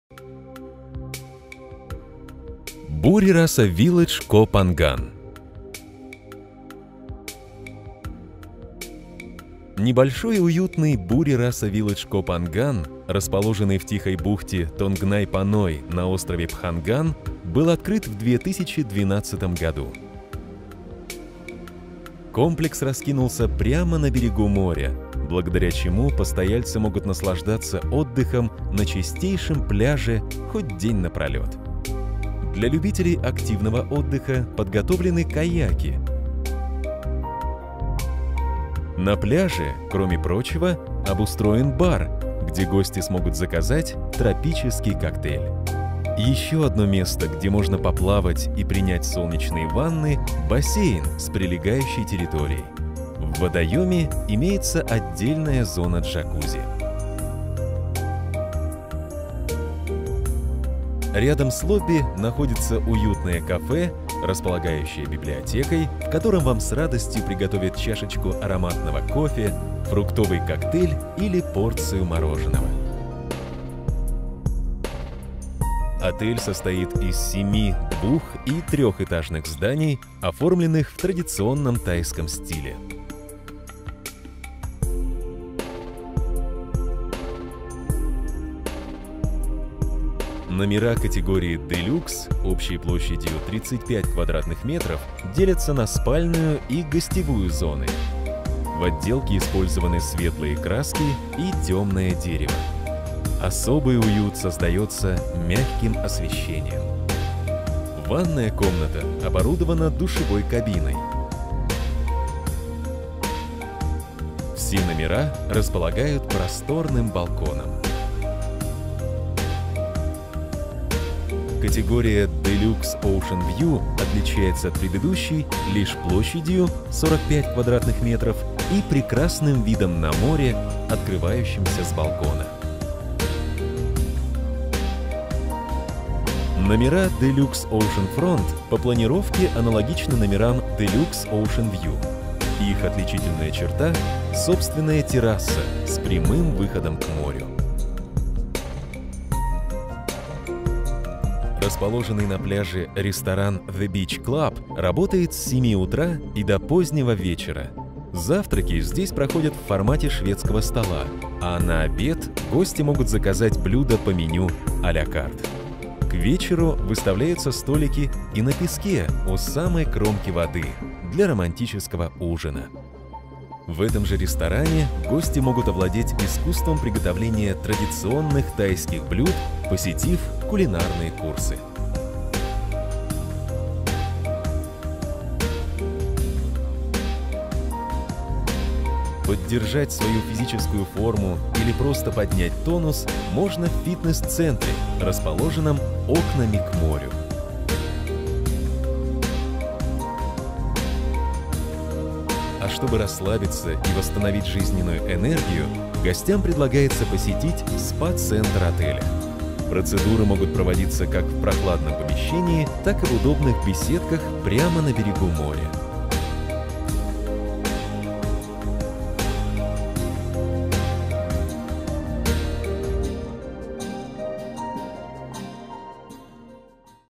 BURI RASA VILLAGE KOH PHANGAN 4*. Лучшие отели Пхангана
Небольшой уютный отель в тихой бухте Тонгнай Паной на острове Пханган был открыт в 2014 году. Дорога от центрального пирса острова, Тонг Сала, куда причаливают паромы с Самуи, занимает 45 минут. В то же время Buri Rasa Village Koh Phangan дает возможность гостям приехать с острова Самуи на скоростном катере напрямую в отель — путешествие длится около 30 минут. Гостиница состоит из семи 2- и 3-этажных зданий, оформленных в традиционном тайском стиле.